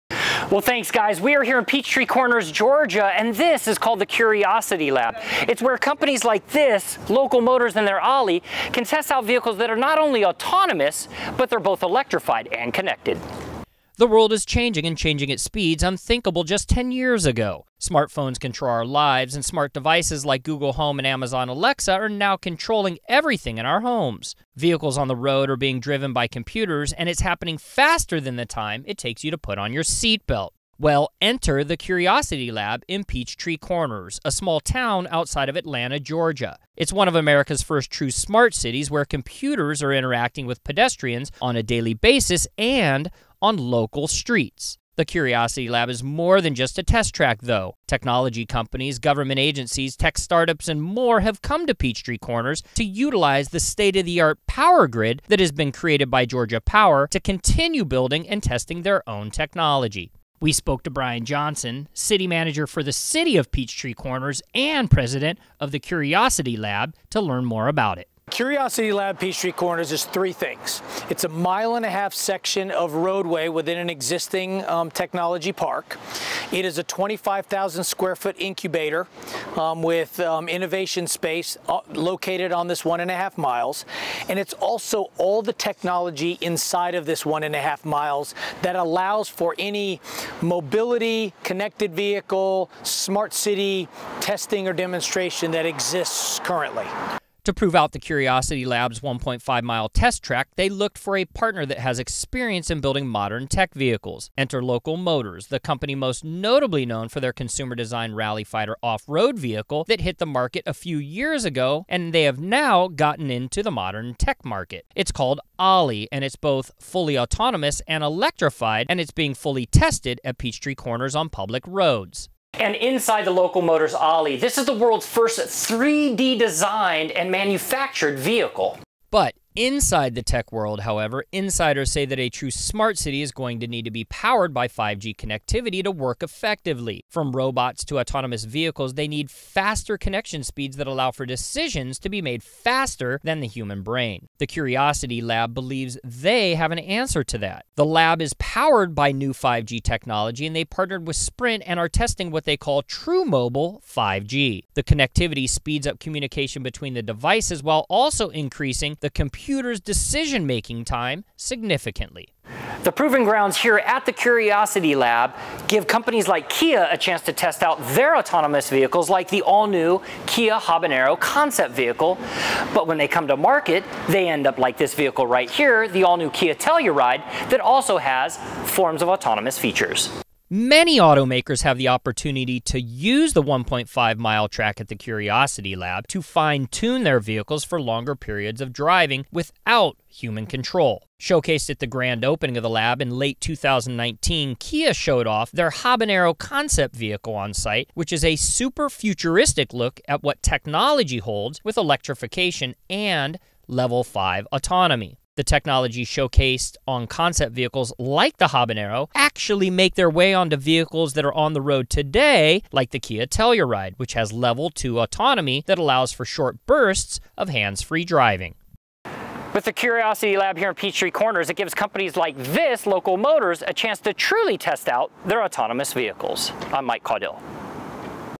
PeachTree Corners
Mike Caudill is in Peachtree Corners Georgia to take a look at the Curiosity Lab where autonomous vehicles are tested